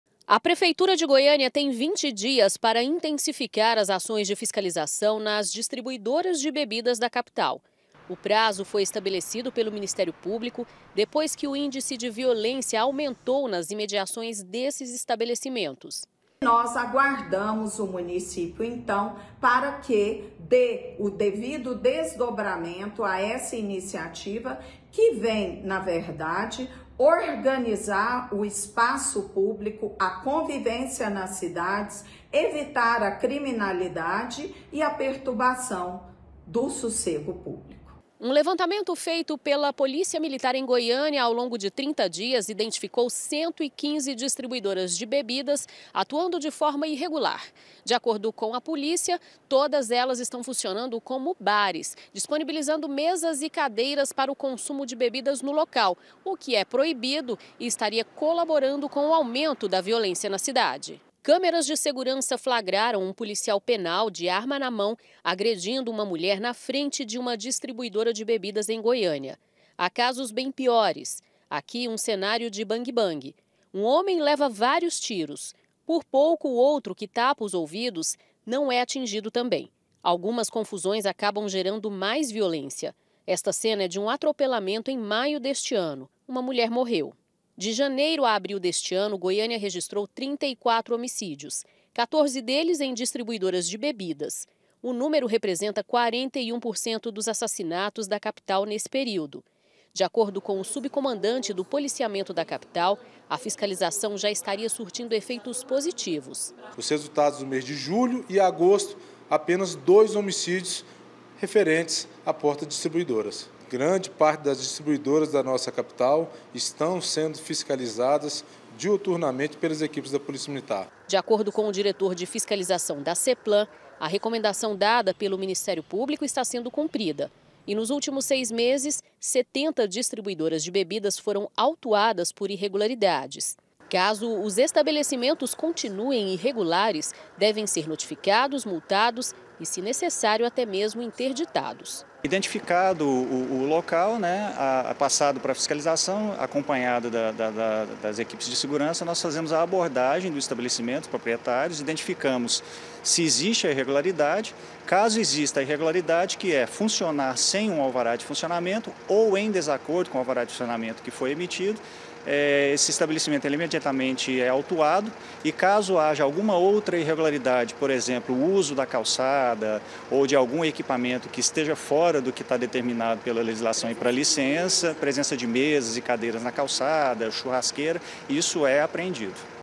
DISTRIBUIDORAS
UMA OPERAÇÃO  DA POLÍCIA MILITAR  DESCOBRIU 115 DISTRIBUIDORAS DE BEBIDAS IRREGULARES  EM GOIÂNIA. ELAS NÃO PODEM FUNCIONAR COMO SE FOSSE UM BAR, UM BOTECO. A GENTE JÁ MOSTROU, AQUI NO GOIÁS RECORD, QUE 41 POR CENTO DOS HOMICÍDIOS EM GOIÂNIA ACONTECEM EM FRENTE A DISTRIBUIDORAS. O MINISTÉRIO PÚBLICO  QUER QUE A FISCALIZAÇÃO SEJA REFORÇADA